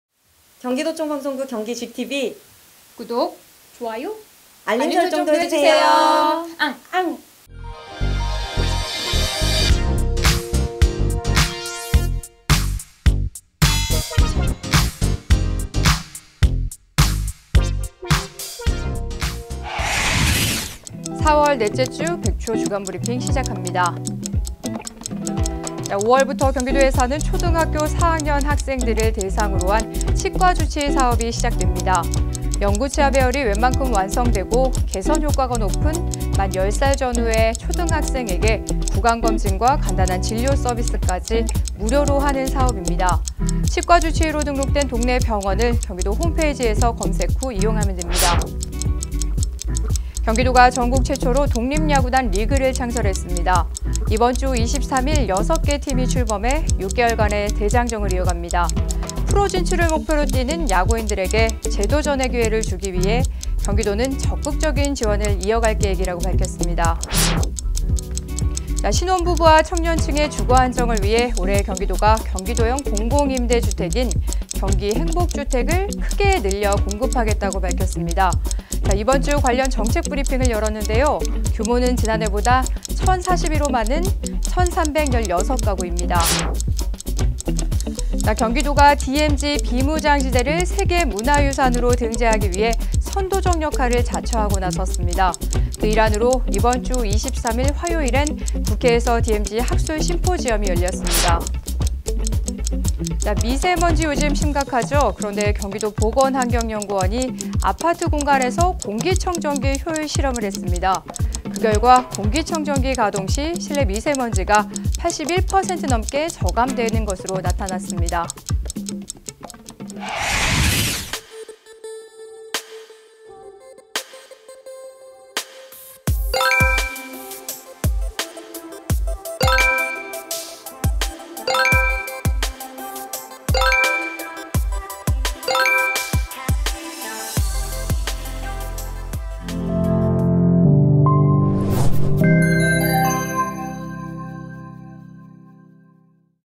100초 주간브리핑 - 4월 넷째 주
4월 넷째 주 경기도의 주요 소식만 간추려!

딱 100초간 전해드립니다.

1. 5월부터 `치과 주치의` 시행…초등4학년 구강치료 지원

2.프로를 향한 ‘희망사다리’…독립야구단 경기도리그 개막

3.경기도형 공공임대주택 ‘경기행복주택’ 올해 1,316가구 공급

4.경기도, DMZ ‘세계문화유산 등재’ 총력

5. “공기청정기 가동 시 미세먼지 81.7% 저감”